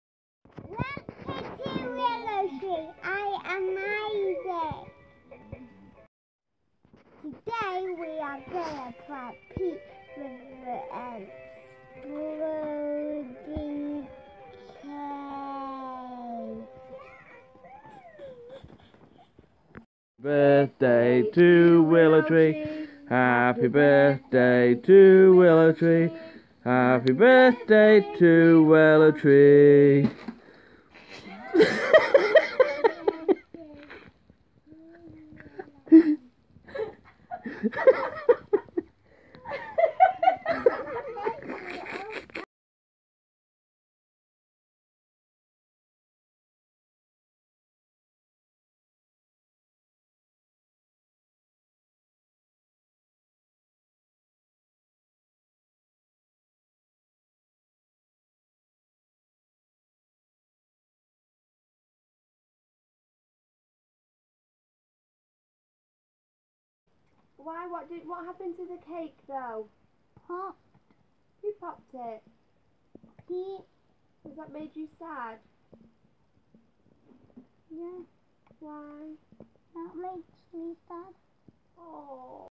The Exploding Cake Prank
So today we decided to prank Uncle Pete.
Pretending we celebrating our 80th video on Willow Tree.. we asked Uncle Pete to cut our cake. our exploding cake... hehehe

Poor Jacob was devastated though.